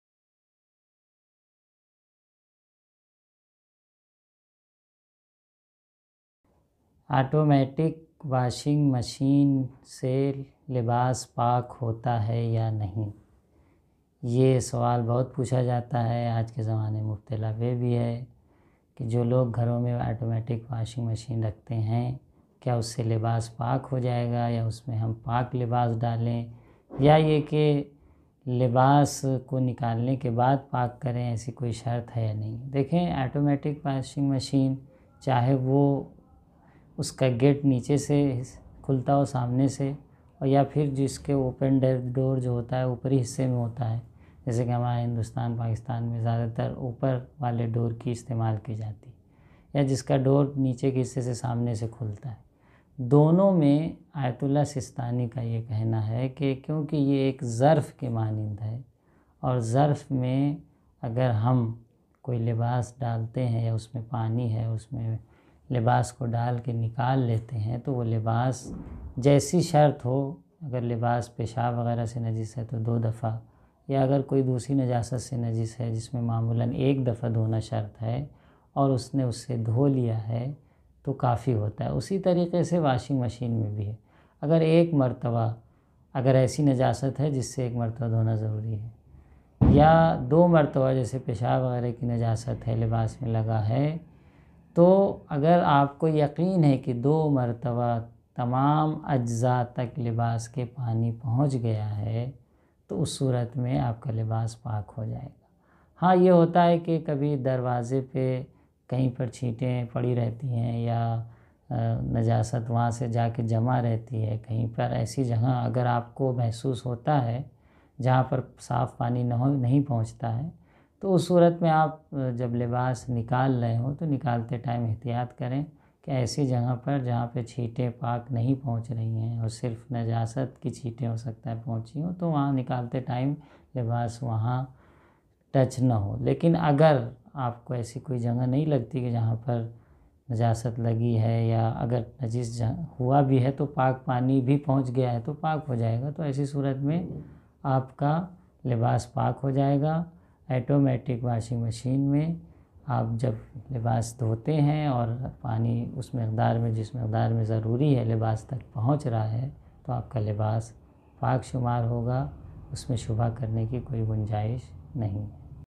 Automatic Washing Machine se NAJIS kapda kaise PAAK Karen? || شرعی مسائل || Payam TV
Shar'ee Masael شرعی مسائل
       ::::::::::::::::: LIVE :::::::::::::::::
Automatic Washing Machine Se Najis Kapda Kaise Pak Karen?

👉 Payam Tv
👉 Gest: Maulana S. Payam Haider Rizvi sb (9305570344)
👉 Production Year:  March 2021